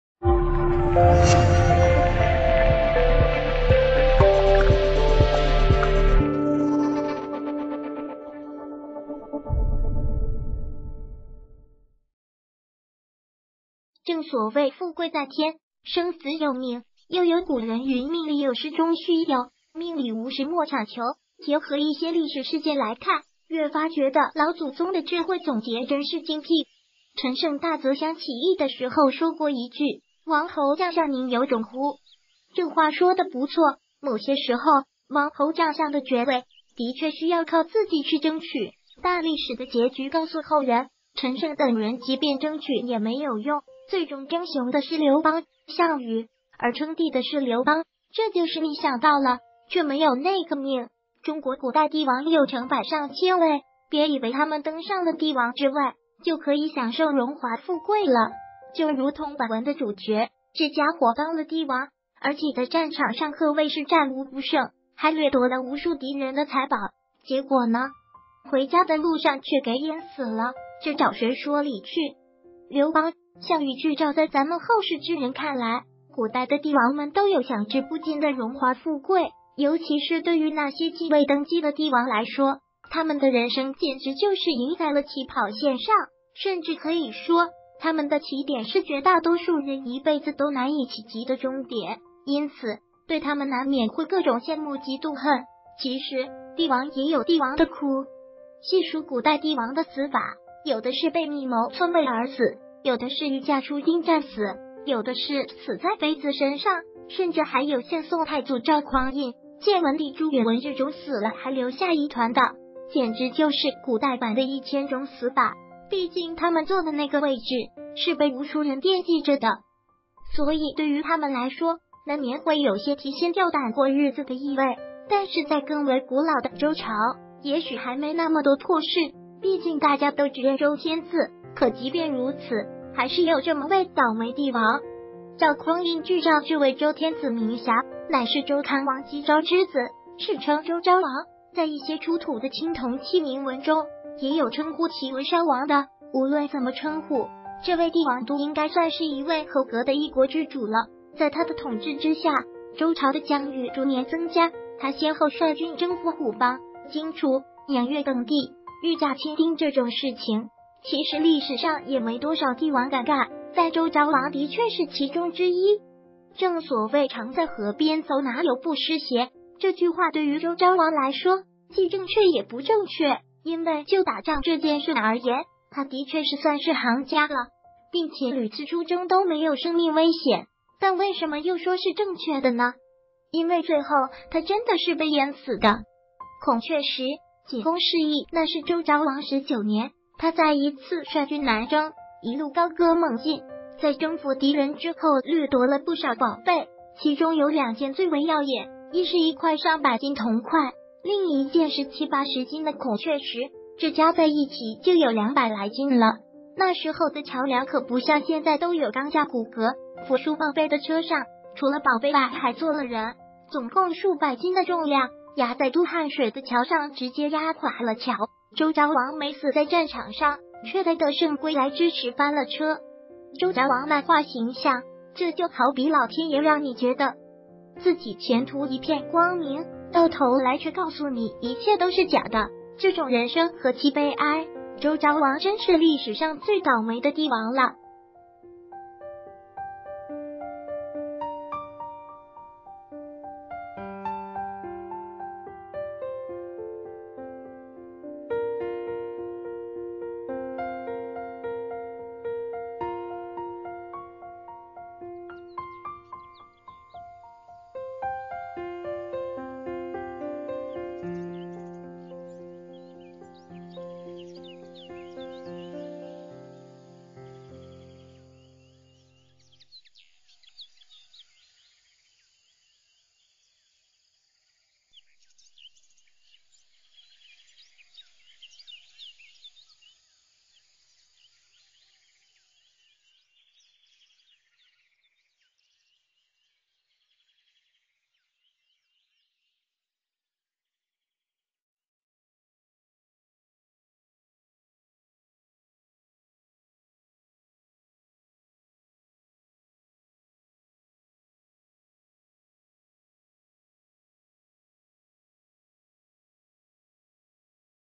古代最倒霉的帝王，打仗抢得两件宝物归来时死了，死因：宝物太重
谢谢！正所谓“富贵在天，生死有命”，又有古人云“命里有时终须有，命里无时莫强求”，结合一些历史事件来看，越发觉得老祖宗的智慧总结真是精辟。陈胜大泽乡起义的时候说过一句：“王侯将相宁有种乎。”这话说得不错，某些时候王侯将相的爵位的确需要靠自己去争取，但历史的结局告诉后人，陈胜等人即便争取也没有用，最终争雄的是刘邦、项羽，而称帝的是刘邦，这就是你“想到了”，却没有那个命，中国古代帝王有成百上千位，别以为他们登上了帝王之位就可以享受荣华富贵了，就如同本文的主角，这家伙当了帝王，而且在战场上可谓是战无不胜，还掠夺了无数敌人的财宝，结果呢？回家的路上却给淹死了，这找谁说理去？刘邦、项羽剧照在咱们后世之人看来，古代的帝王们都有享之不尽的荣华富贵，尤其是对于那些继位登基的帝王来说，他们的人生简直就是赢在了起跑线上，甚至可以说他们的起点是绝大多数人一辈子都难以企及的终点，因此对他们难免会各种羡慕嫉妒恨，其实帝王也有帝王的苦。细数古代帝王的死法，有的是被密谋篡位而死，有的是御驾出征战死，有的是死在妃子身上，甚至还有像宋太祖赵匡胤、建文帝朱允炆这种死了还留下疑团的，简直就是古代版的“一千种死法”，毕竟他们坐的那个位置是被无数人惦记着的，所以对于他们来说难免会有些提心吊胆过日子的意味，但是在更为古老的周朝，也许还没那么多破事，毕竟大家都只认“周天子”，可即便如此，还是有这么位倒霉帝王。赵匡胤剧照这位周天子名瑕，乃是周康王姬钊之子，史称周昭王，在一些出土的青铜器铭文中也有称呼其为卲王的，无论怎么称呼，这位帝王都应该算是一位合格的一国之主了，在他的统治之下，周朝的疆域逐年增加，他先后率军征服虎方、荆楚、扬粤等地，御驾亲征这种事情其实历史上也没多少帝王敢干，但周昭王的确是其中之一。正所谓“常在河边走哪有不湿鞋”，这句话对于周昭王来说既正确也不正确，因为就“打仗”这件事而言，他的确是算是行家了，并且屡次出征都没有生命危险，但为什么又说是正确的呢？因为最后他真的是被淹死的。孔雀石，仅供示意那是周昭王十九年，他再一次率军南征，一路高歌猛进，在征服敌人之后掠夺了不少宝贝，其中有两件最为耀眼，一是一块上百斤铜块，另一件是七八十斤的孔雀石，这加在一起就有两百来斤了，那时候的桥梁可不像现在都有钢架骨骼，运输宝贝的车上除了宝贝外还坐了人，总共数百斤的重量压在渡汉水的桥上直接压垮了桥，周昭王没死在战场上，却在得胜归来之时翻了车。周昭王漫画形象这就好比老天爷让你觉得自己前途一片光明，到头来却告诉你一切都是假的，这种人生何其悲哀，周昭王真是历史上最倒霉的帝王了。